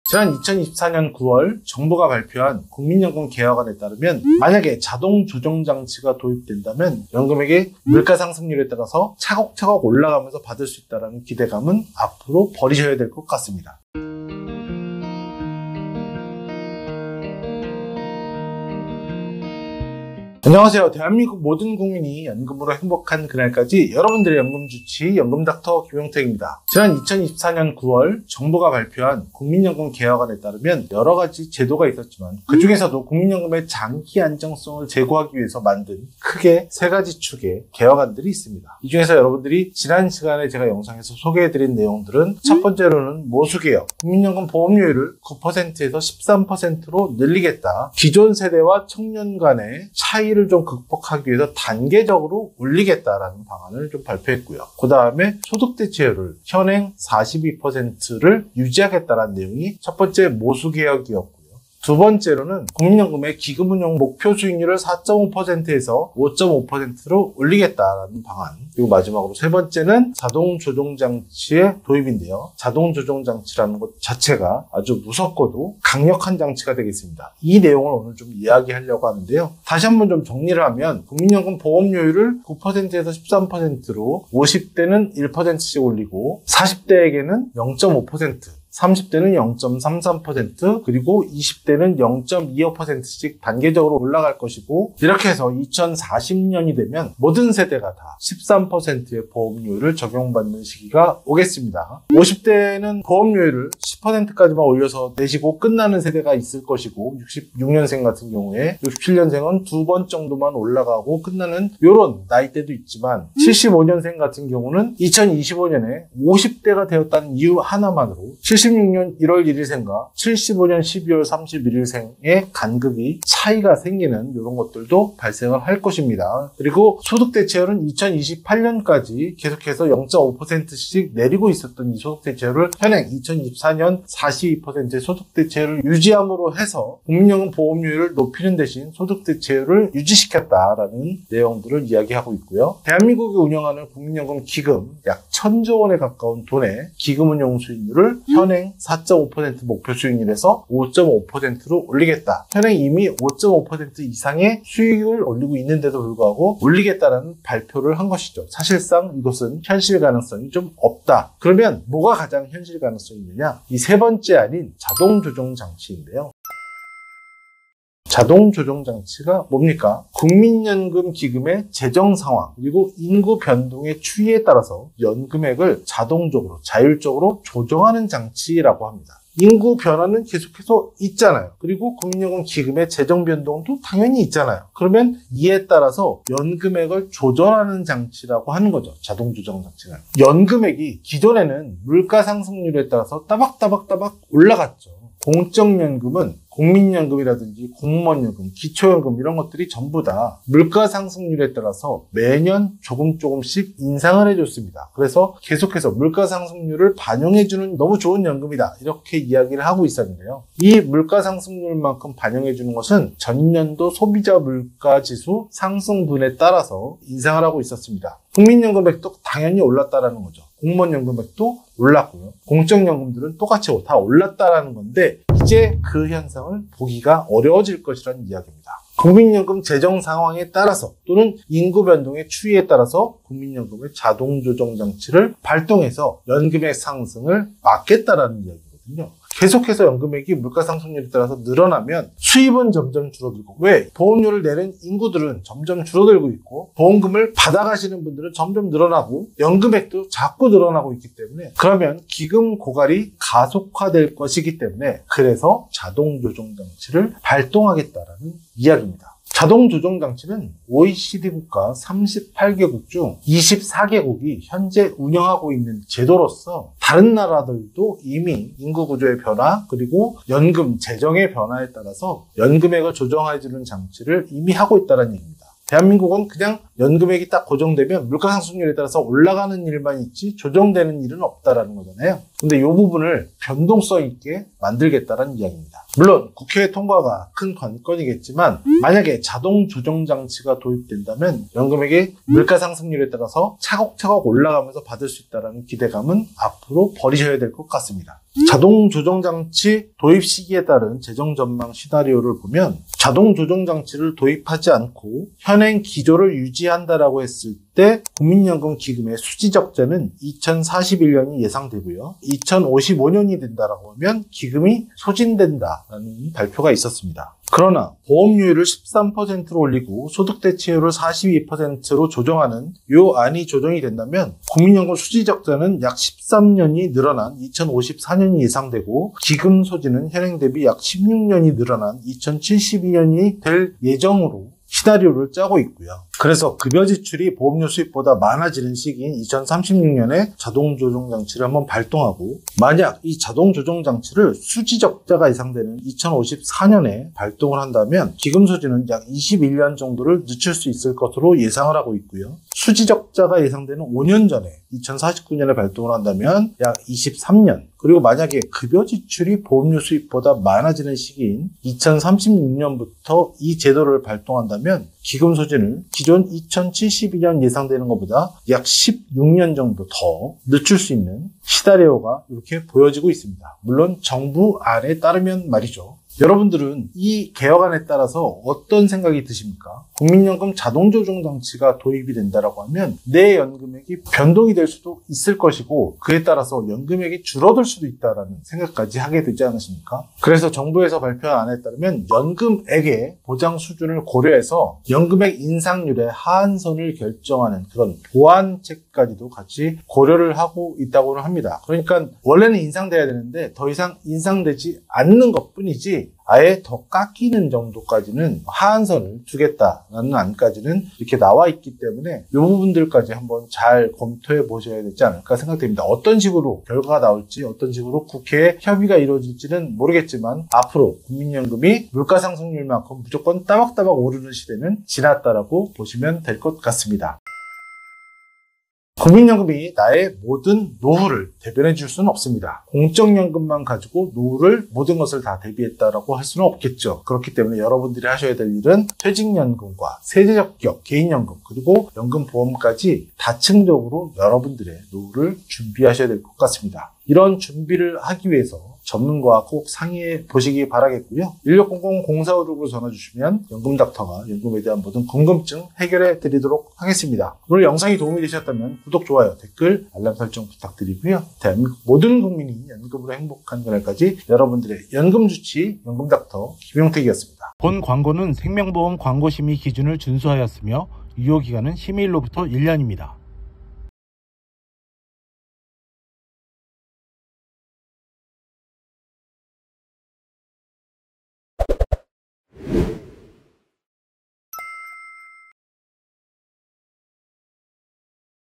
국민연금 자동조정장치 도입 초읽기, 사실상 연금 삭감되는건 아닌지 걱정됩니다
💭 연금에 대한 궁금증이 있으시거나
상담을 원하시는 분들은 주저하지
마시고 아래 연락처로 연락주시기 바랍니다.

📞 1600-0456
📧 아래의 SNS 채널들에서 소통가능합니다. [링크참고 📌]

🔽 영상목차 🔽
00:00 하이라이트
00:13 인트로
02:47 자동조정장치
07:07 내용정리